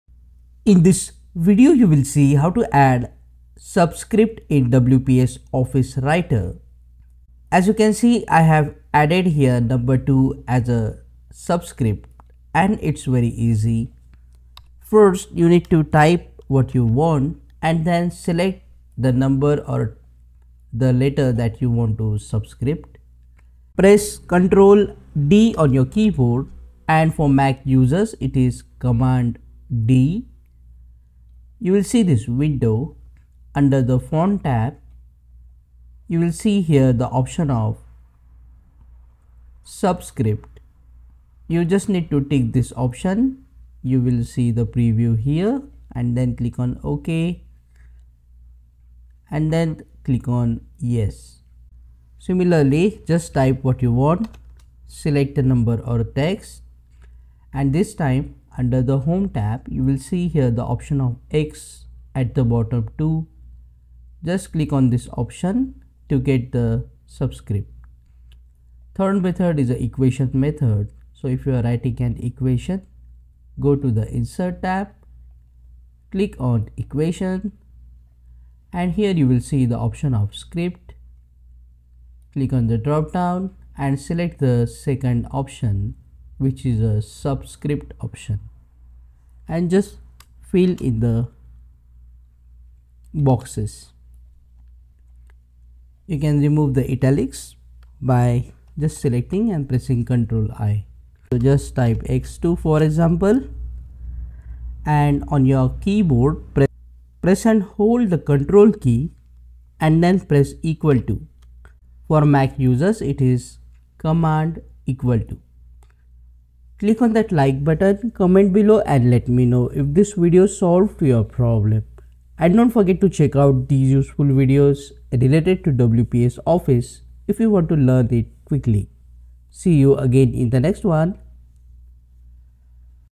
How To Add Subscript In WPS Office
How To Add Subscript In WPS Office Writer and write H2O Co2 etc is shown in the video